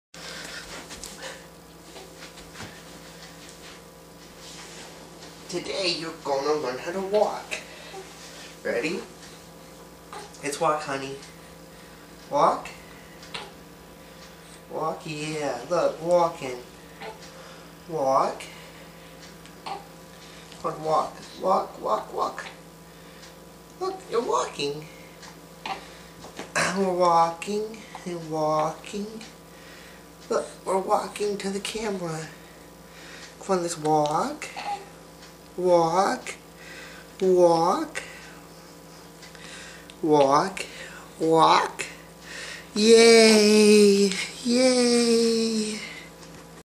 Charlie practicing walking!
Daddy teaching Charlie to walk